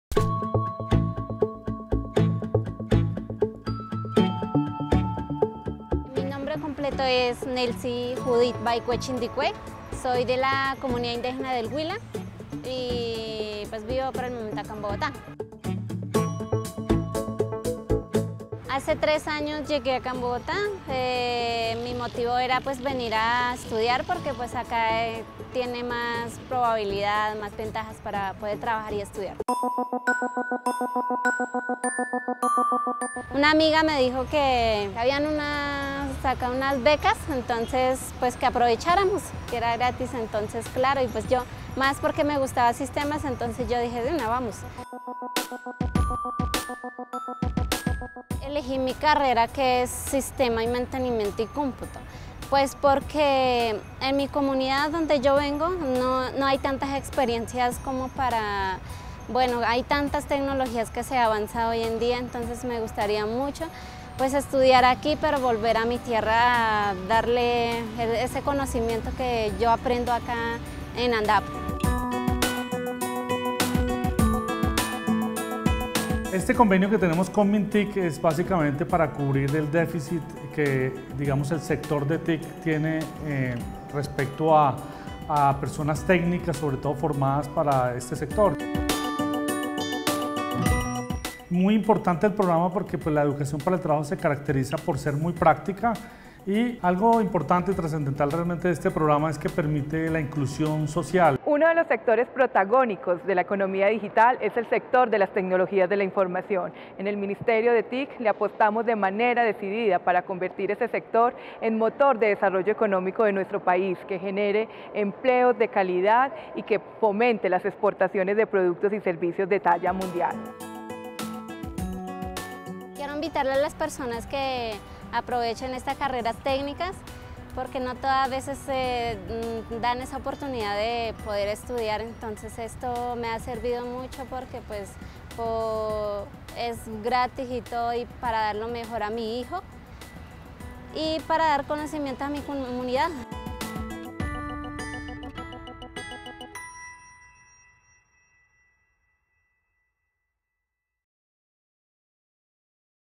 Talento TI, apoyando la formación de los jóvenes colombianos C30-N4
Jóvenes de diferentes regiones se benefician de las becas de Talento TI para ser los futuros profesionales de la tecnología al servicio de su gente.